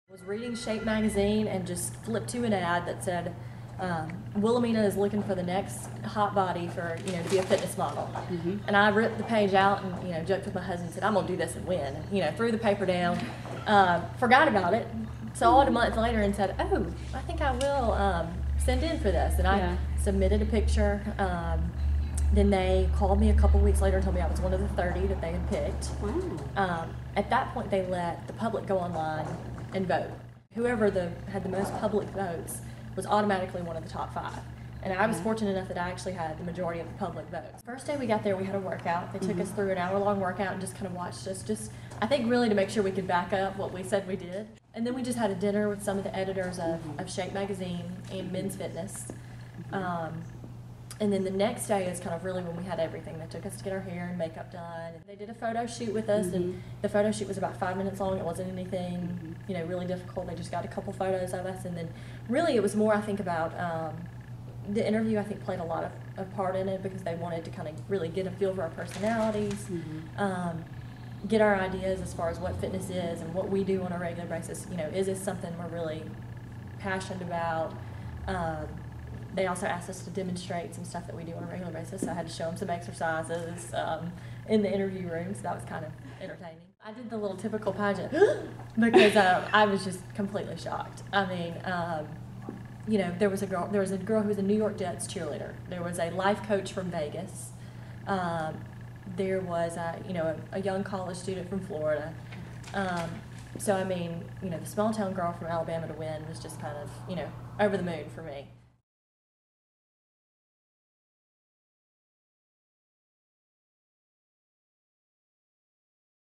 Keisha Brazell wins Wilhemina model Search
Brazell, co-owner of CrossFit Embracd wins Wilhemina Hot Body Model
Search. She has a five-year modeling contract and will be in the June
issue of Shape magazine